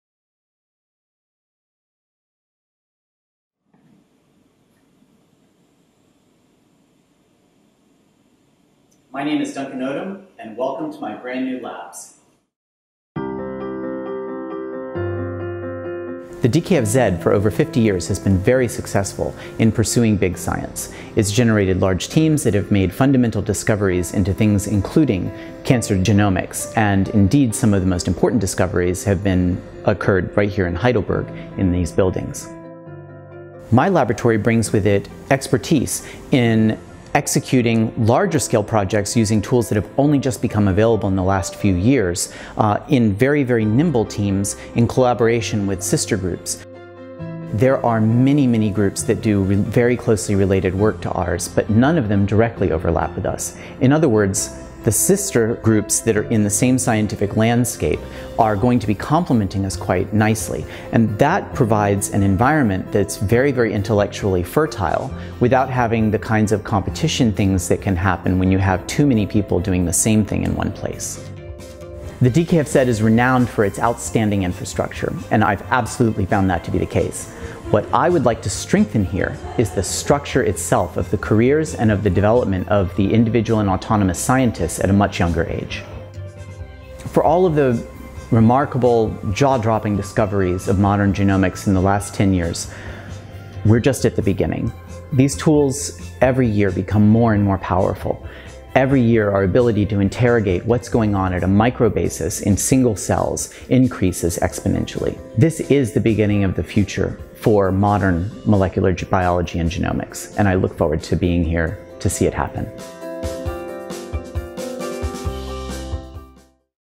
Neue Teamstrukturen für große Forschungsprojekte - ERC Grant Holder Duncan Odom, DKFZ
Duncan Odom forscht auf dem Gebiet der Krebsgenomik am DKFZ. Sein Projekt „CTCFStableGenome“ wird durch einen ERC Advanced Grant gefördert. Er und seine Abteilung bringen eine Expertise mit, größere Projekte im Bereich der Krebsgenomik durchzuführen mithilfe neuer und modernster Methoden, die erst seit wenigen Jahren zur Verfügung stehen. Dazu ist die Kooperation mit anderen Teams sehr wichtig. So ergänzen sich Labore, die an ähnlichen Projekten arbeiten, ohne sich gegenseitig Konkurrenz zu machen. Die Infrastruktur des DKFZ begünstigt diese Arbeitsweise.